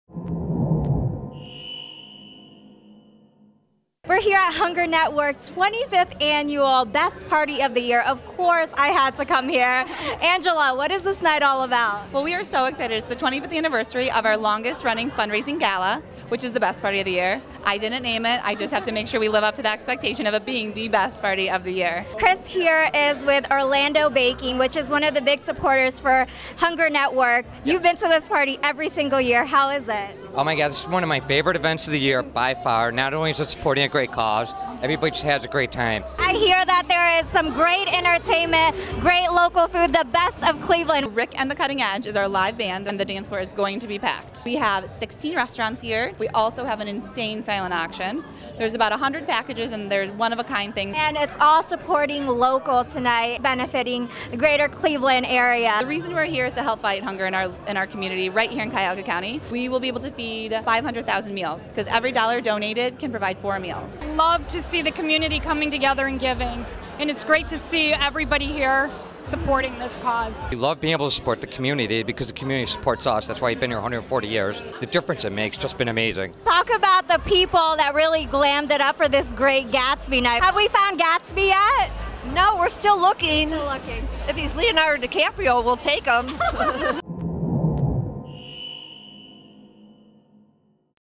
Hunger Network celebrates 25th annual "Best Party of the Year"
Watch as the Hunger Network Of Greater Cleveland celebrates their 25th annual &quot;Best Party of the Year&quot; at the Renaissance Hotel Cleveland on Friday. The Great Gatsby-themed night helps raise money to feed hungry families in Cleveland.